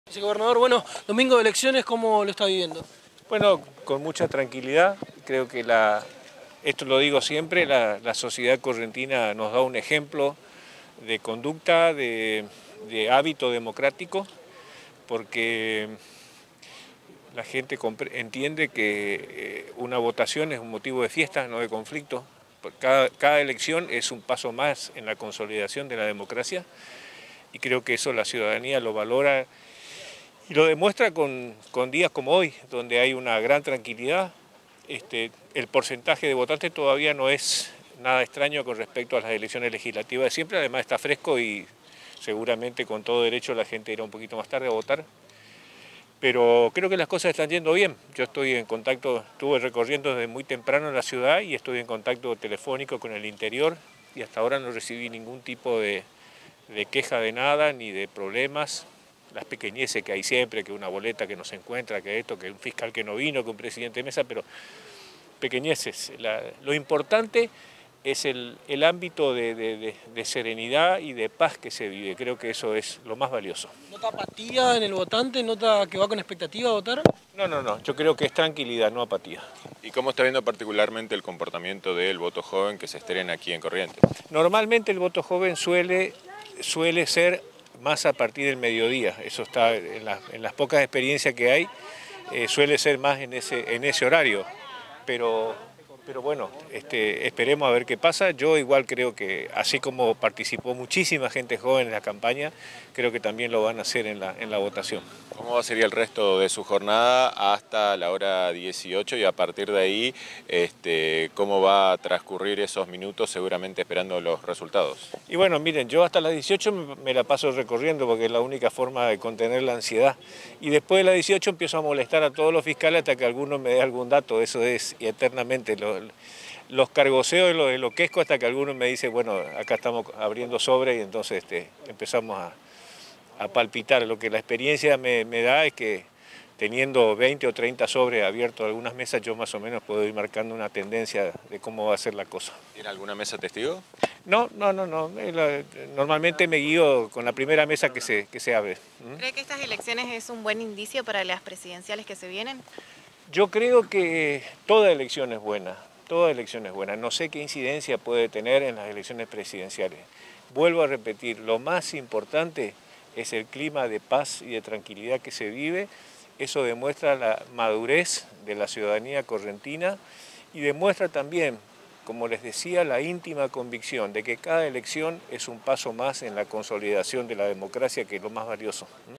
BRAILLARD POCCARD SOSTUVO QUE “TODA ELECCIÓN ES BUENA” Y MUESTRA “LA MADUREZ DE LOS CORRENTINOS”
El vicegobernador Pedro Braillard Poccard emitió su voto este mediodía, en la mesa N°4 de la Escuela Centenario, de Capital. “Cada elección es un paso más en la consolidación de la democracia”, consideró el mismo, a la vez que destacó el compromiso de la sociedad correntina, en cuanto a su compromiso ciudadano, en estos 40 años de democracia.
“La sociedad correntina nos da un ejemplo de conducta en su hábito democrático”, ya que “la gente entiende que una votación es un motivo de fiesta, no de conflicto”, comenzó el vicegobernador, al ser consultado por la prensa.
Braillard Poccard expresó, en la oportunidad, que “cada elección es un paso más en la consolidación de la democracia, y lo importante es el ámbito de serenidad y paz que se vive en la jornada”.
Respecto al estreno y participación del Voto Joven, el mismo vislumbró que la participación juvenil “se da más hacia el mediodía”, vaticinando una buena concurrencia, ya que “muchísima gente joven participó durante la campaña”.
CÁMARA-EDICIÓN: MARCOS ALARCÓN